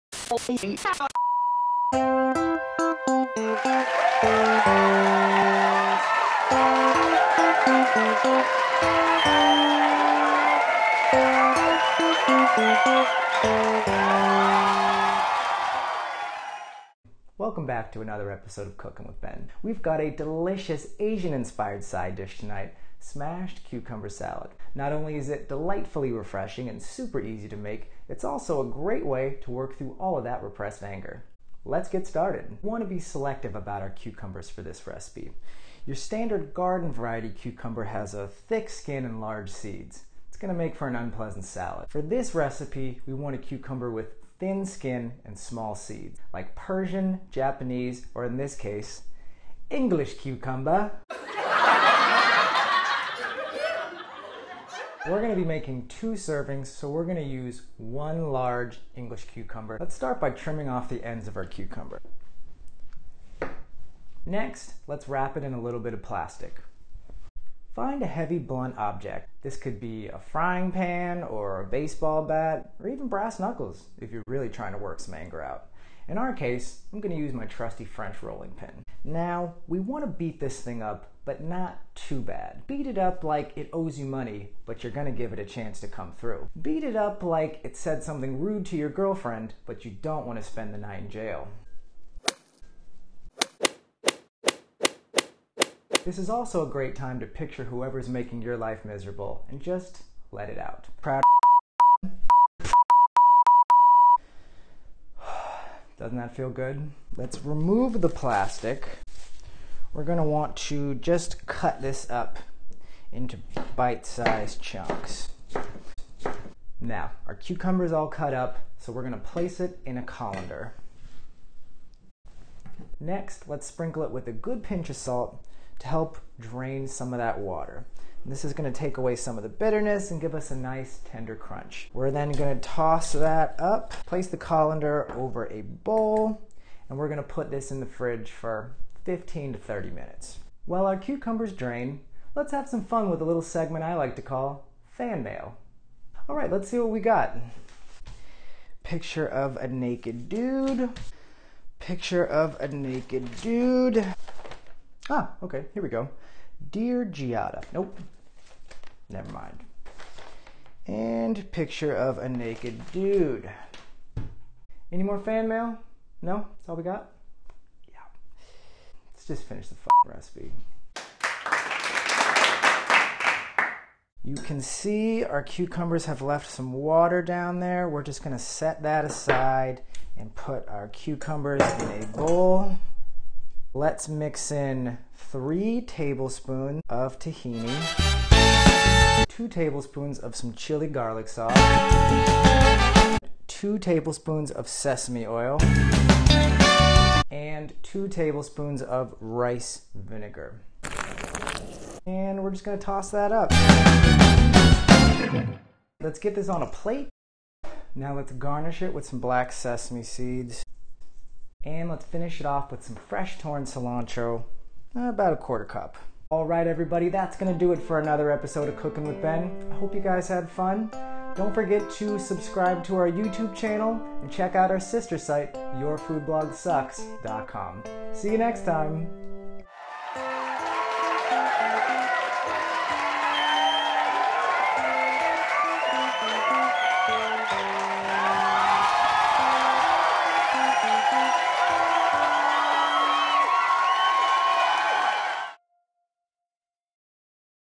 Spicy Smashed Cucumber Salad
This recipe is one of my personal favorites, combining my love of delicious food with the childhood barbarian side of me that likes to smash things. If you like to cook, but also have a lot of repressed anger you'd like to get out, then this recipe is for you.

-Ben